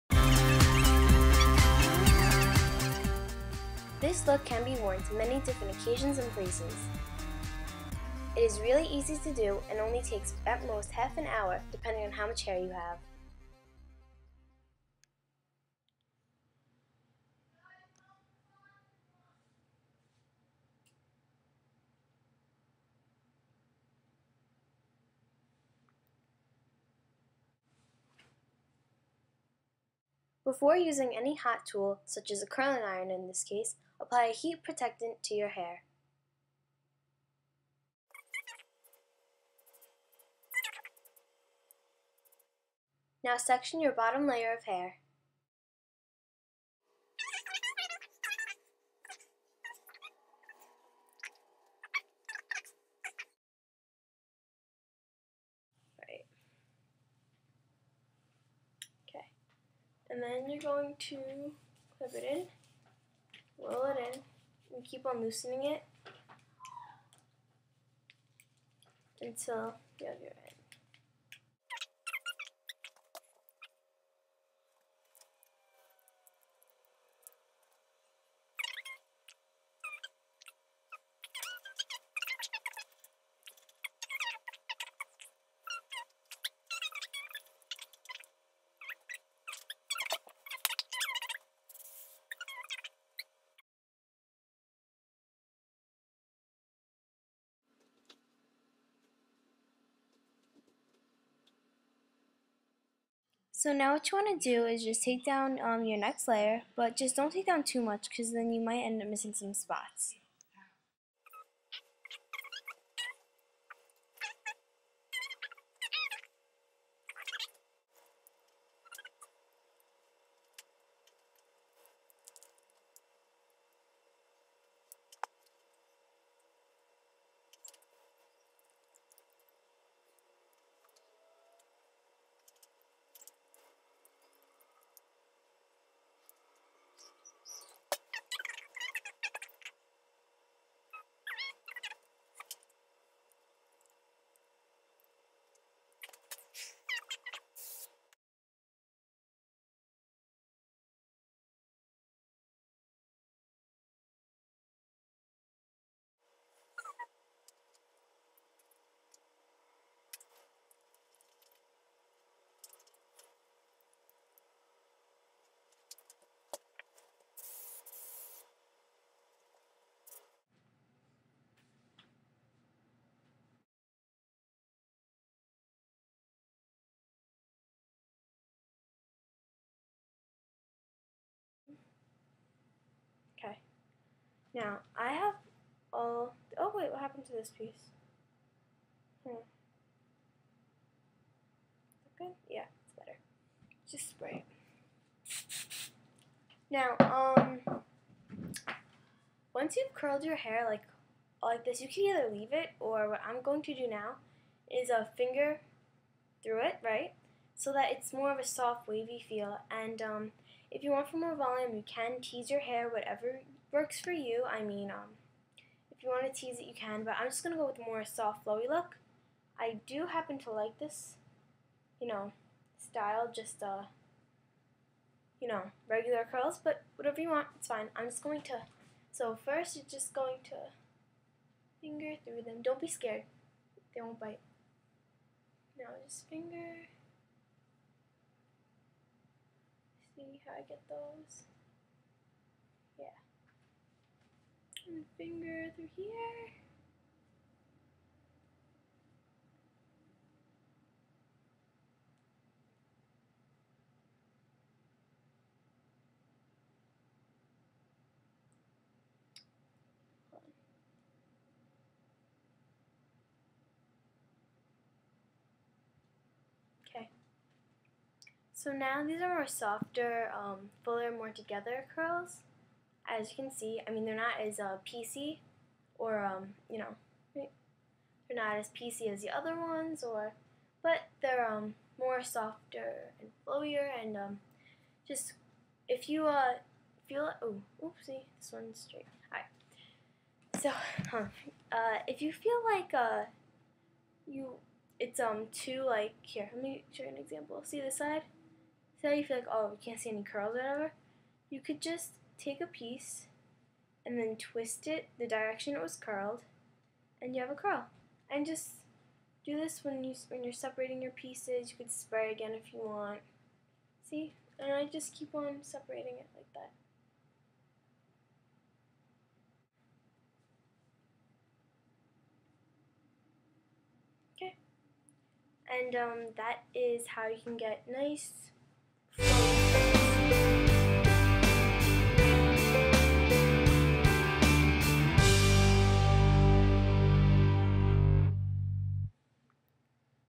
How To Curl Your Hair! easy & simple!
Here is a tutorial on easy flowy curls! hope you like!:)